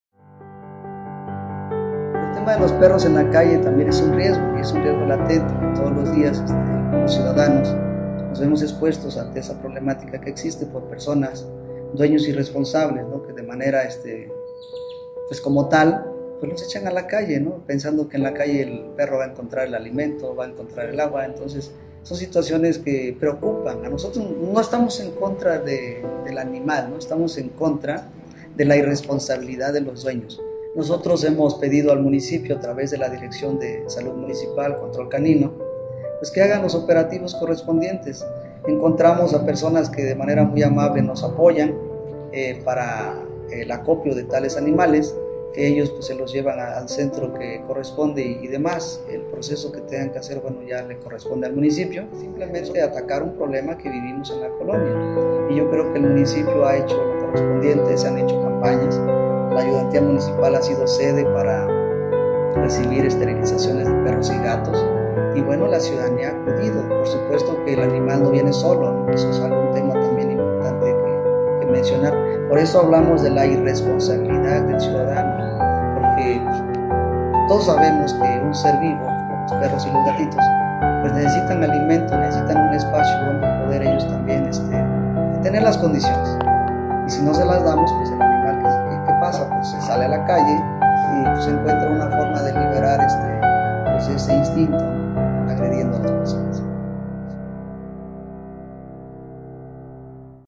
El incremento de perros callejeros en Cuautla.
Pese a los riesgos de salud pública que ocasiona el incremento de perros callejeros en Cuautla, no hay acciones encaminadas a minimizar la situación y buscar el bienestar de estos seres vivos.